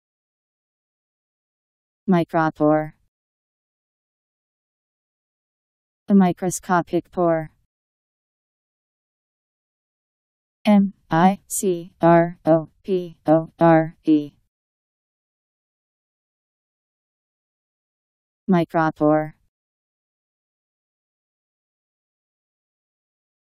Micropore Meaning
Video shows what micropore means. A microscopic pore.  Micropore Meaning. How to pronounce, definition audio dictionary. How to say micropore. Powered by MaryTTS, Wiktionary